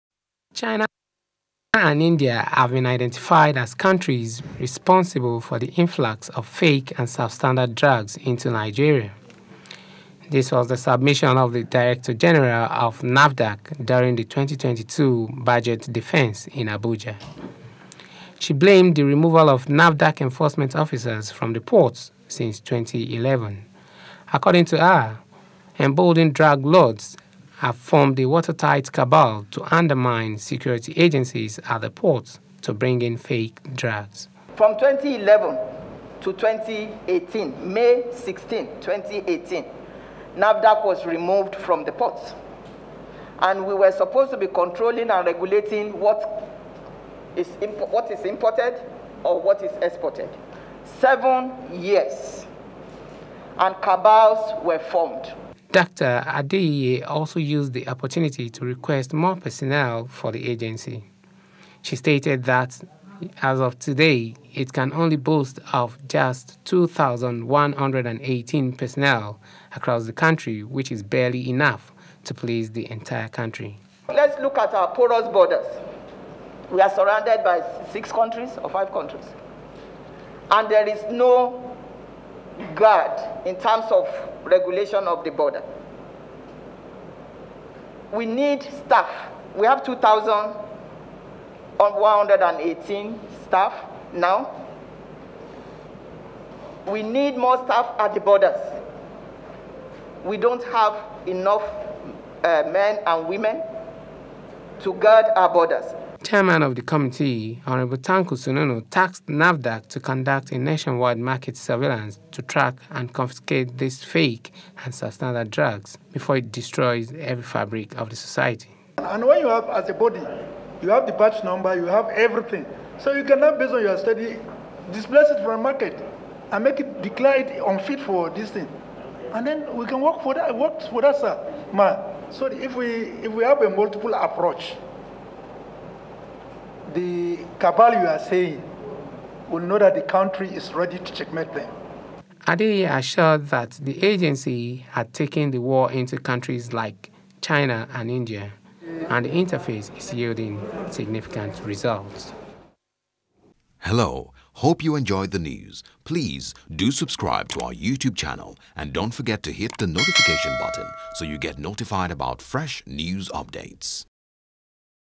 Fake Drugs: China, India Responsible For 95% Influx In Nigeria - N.A.F.D.A.C Boss | NEWS
The Director-General of N.A.F.D.A.C, Dr. Mojisola Adeyeye has hinted that the commission is in need of personnel to combat drug trafficking in the country.